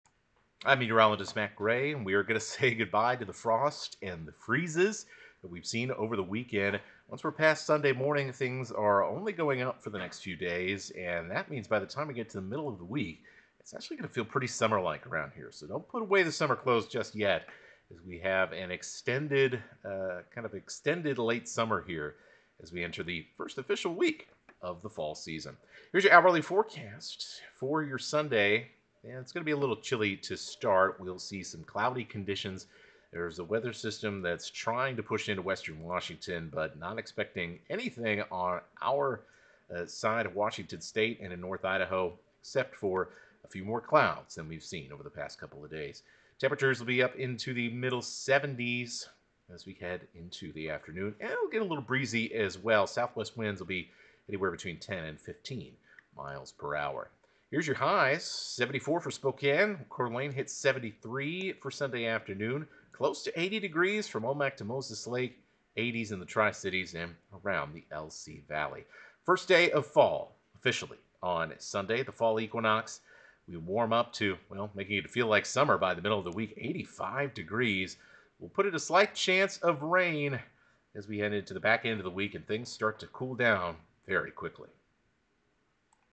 Warming up Sunday with summer-like weather ahead
It was frosty this weekend, but be ready for summer warmth to return.